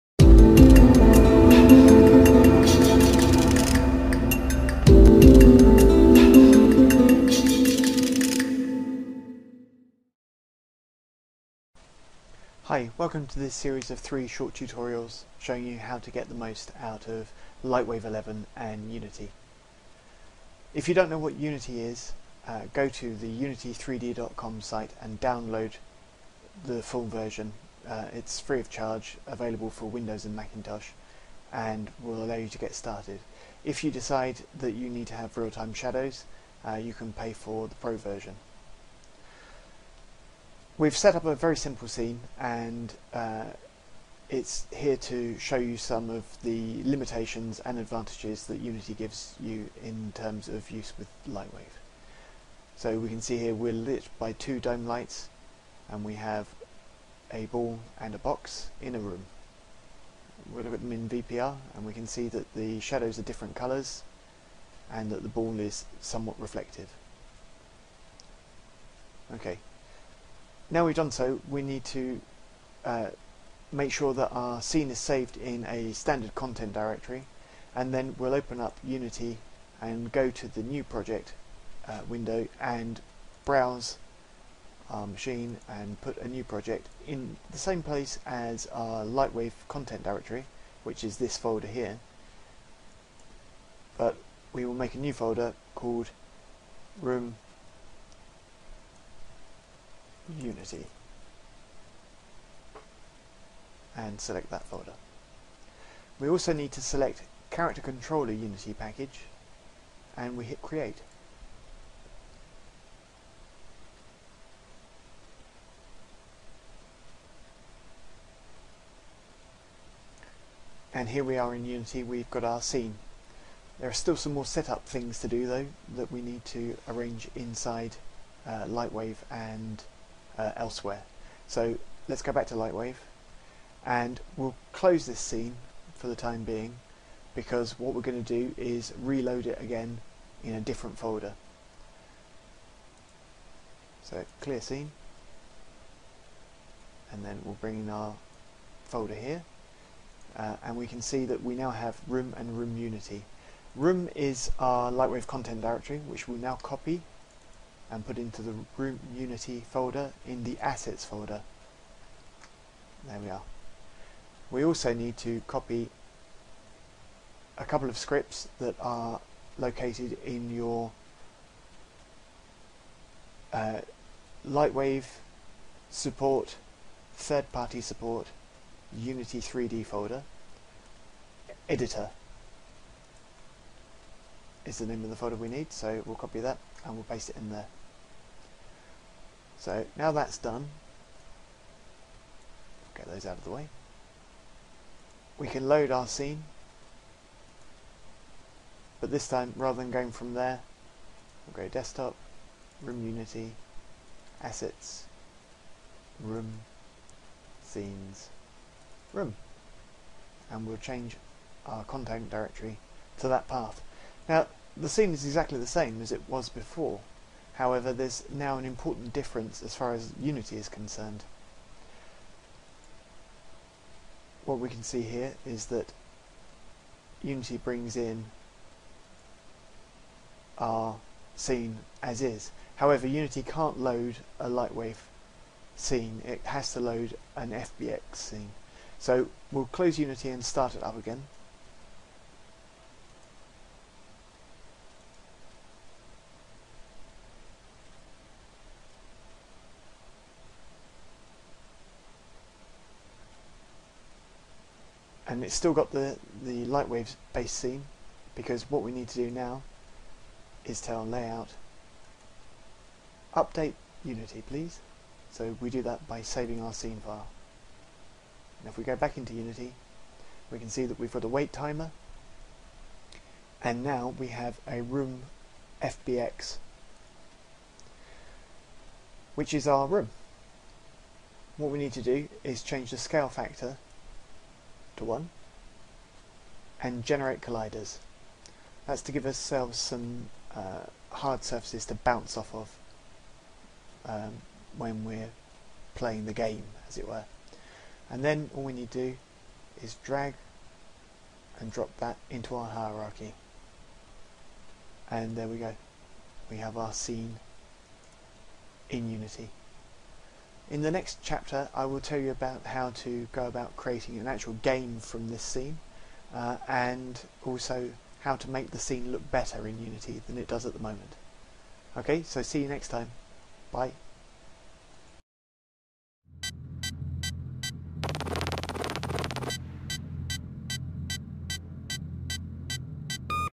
LightWave 11 Unity Workflow Part 1
Part 1 of a 3 part tutorial series with Ben Vost explaining the LightWave 11 to Unity workflow.